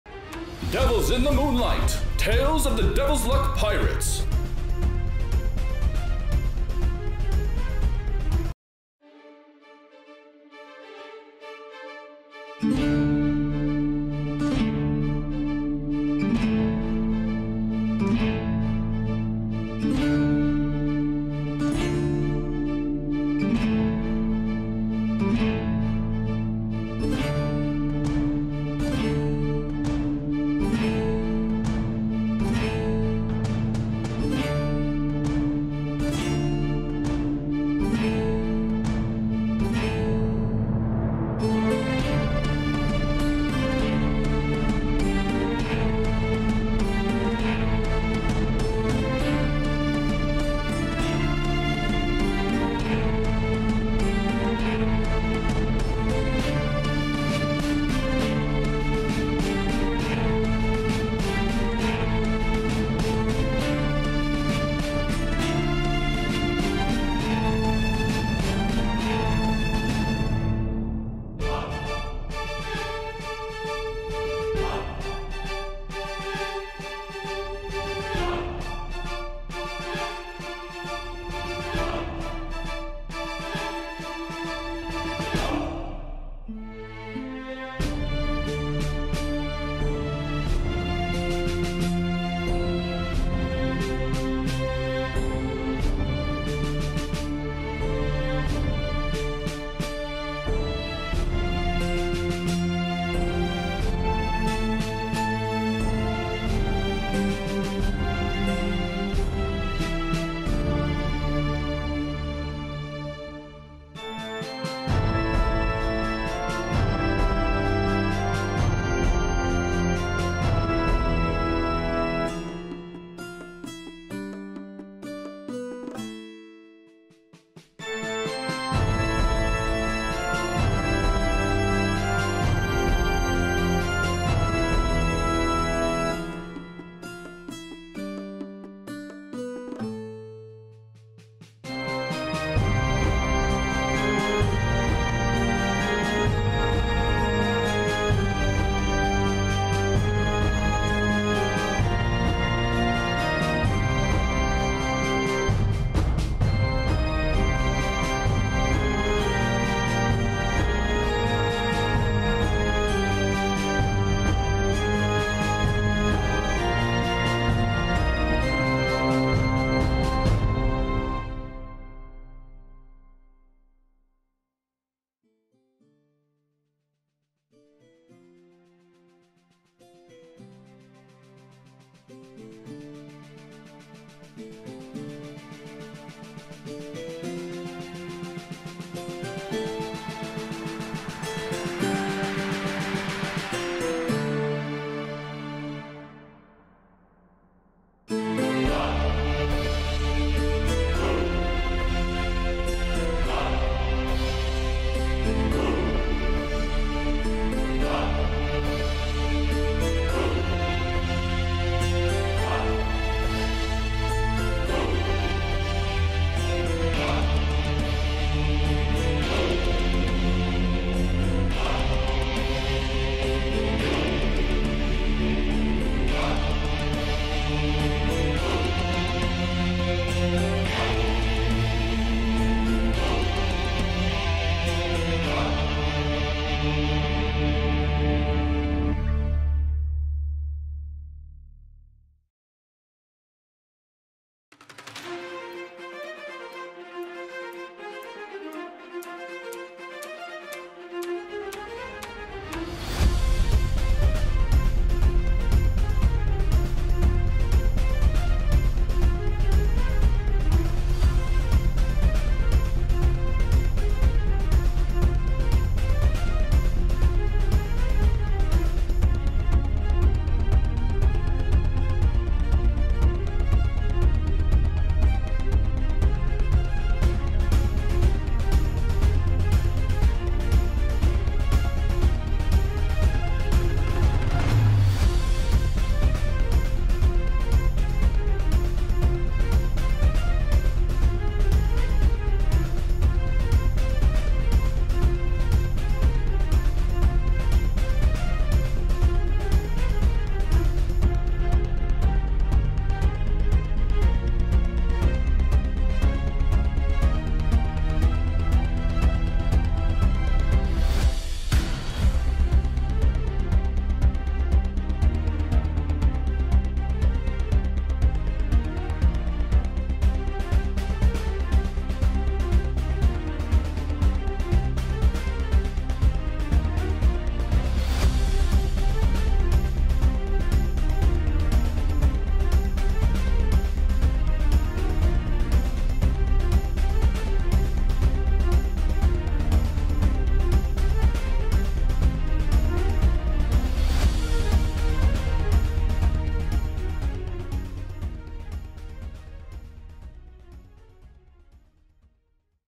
One Piece D&D MANGA TIME LAPSE | Chapter 13 Cover Page | DEVILS IN THE MOONLIGHT
Based on the D&D series by RUSTAGE | William, Duros, Ragnar, and Verona took to the seas as pirates. After a shipwreck wipes out most of their crew, they meet a mysterious girl, named Pearl. This makeshift group of people, brought together by misfortune, sets out to sea seeking answers to both their individual quests and the meaning of a crew.

Other links
Wiki:

Art by Bad Apostrophe

This Manga is UNOFFICIAL and is in no way supported by RUSTAGE, Tekking101, Briggs, 2Spooky, Lost Pause, or Kaerru